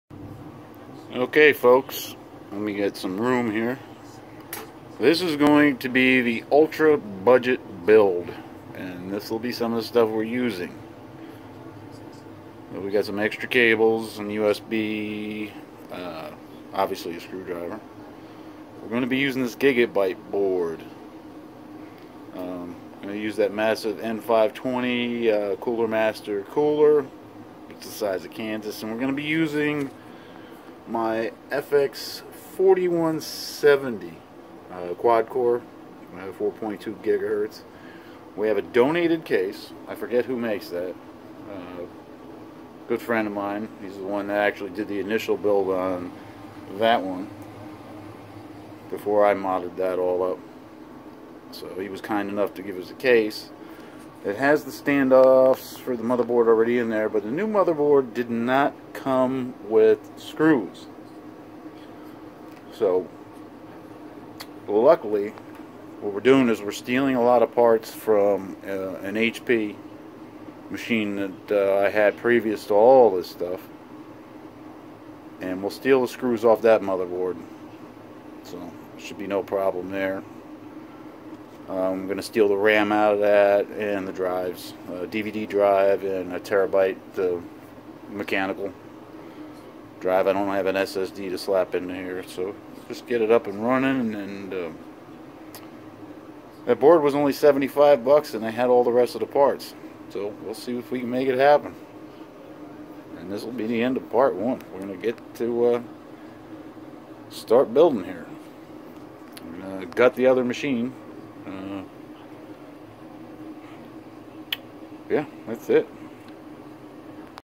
PC Ultra Budget Build (Part 1)
This just pretty much tells you what we're working with. In a nutshell, A donated case, FX-4170 CPU, Gigabyte 970 MB, EVGA GTX460 V2 GPU, 8 gig Gskill RipjawX, 500 watt Rosewill PSU. The two drives and hardware came out of an HP we broke down. Had a couple blue LED fans too hanging around. As for the CPU cooler, we couldn't use the CM Hyper N520. WAY too big! Ended up using the, "Wraith" cooler which came with my FX-8350. You'd be surprised at what we had to come up with on the fly!